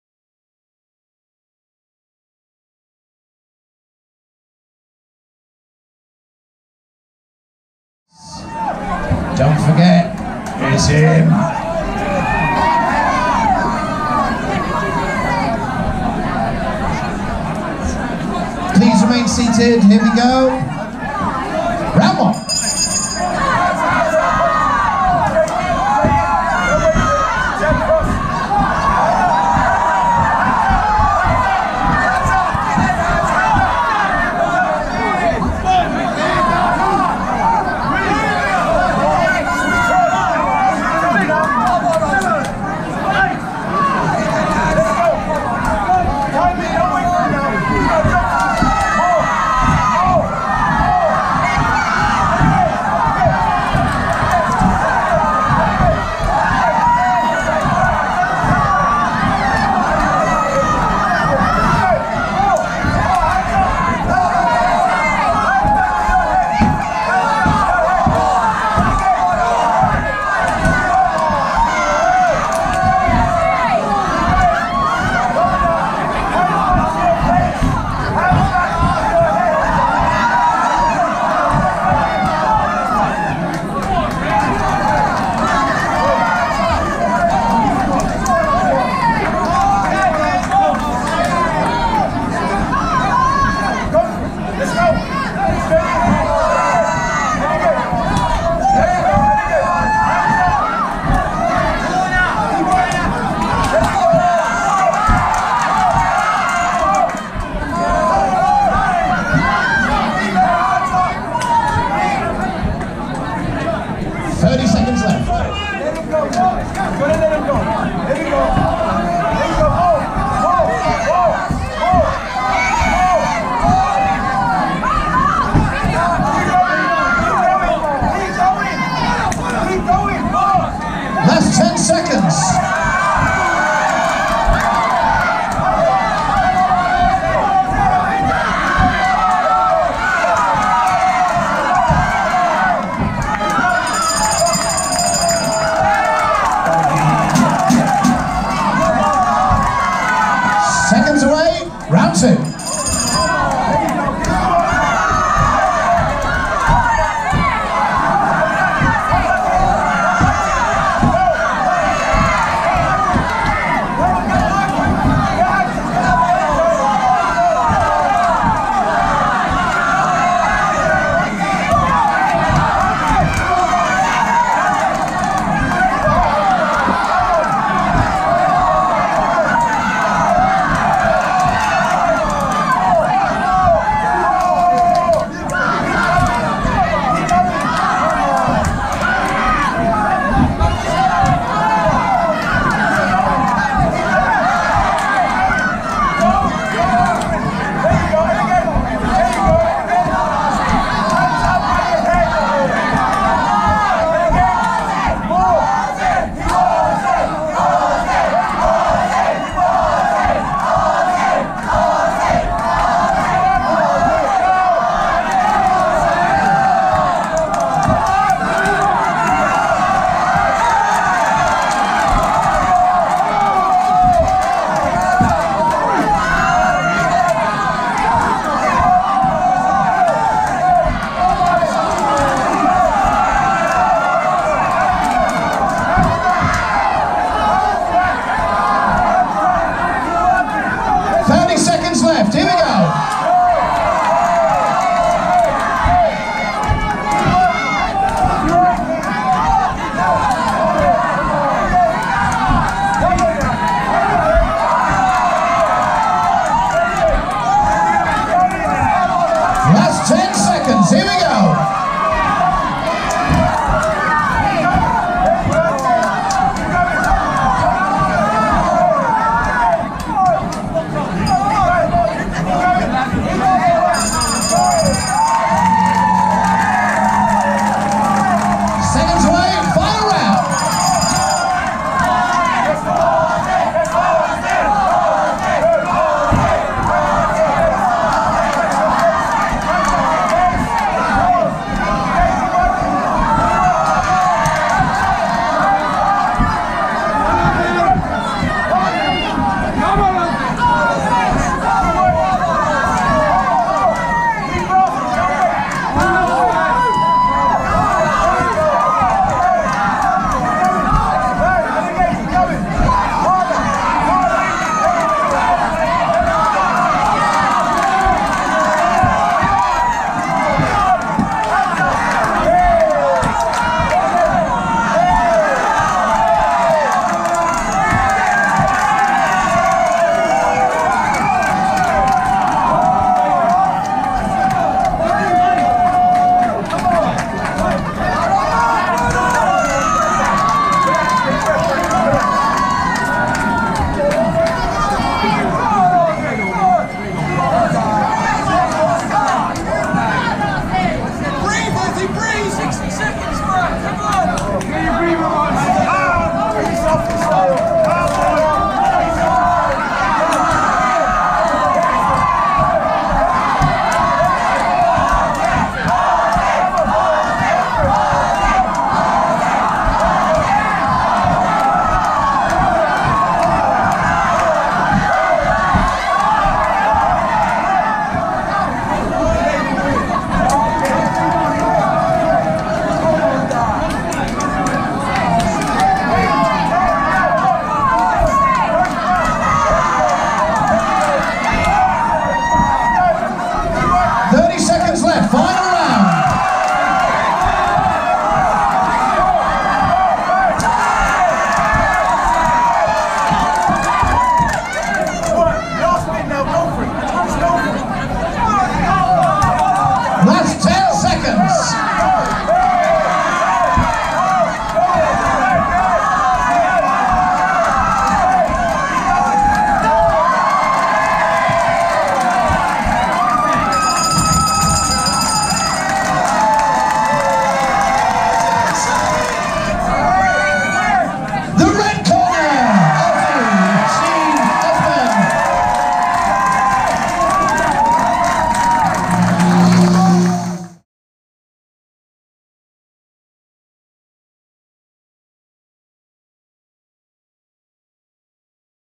Ultra White Collar Boxing | Ipswich | Ozzy Osman VS Tyler Holland
Ipswich Ultra White Collar Boxing 30th March 2019

8 Weeks FREE training / Get in the best shape of your life/ Raise money for Cancer Research UK